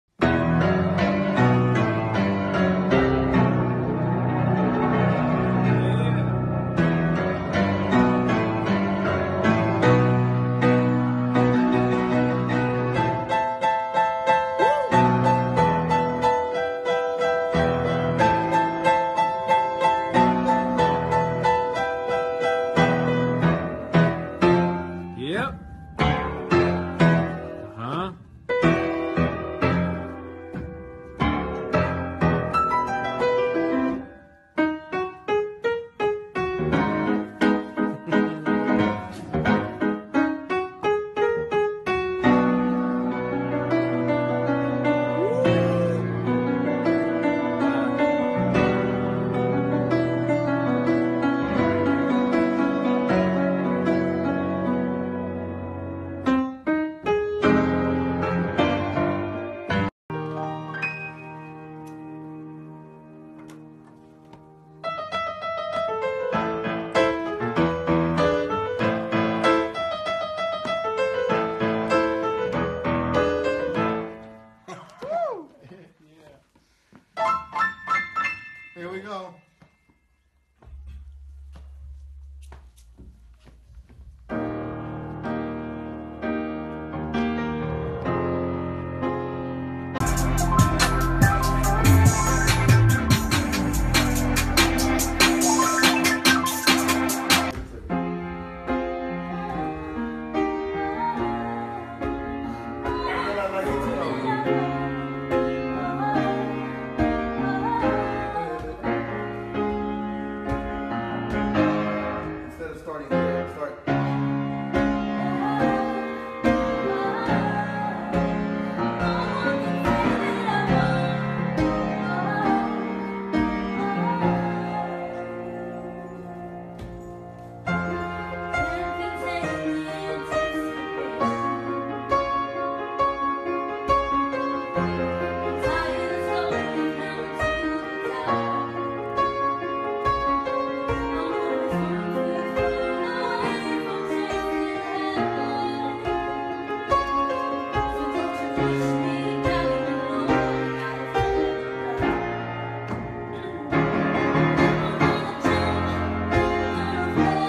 Scott Storch Playing Some Of His Classic Beats On The Piano
Scott Storch Plays dre dre songs and some of his classics.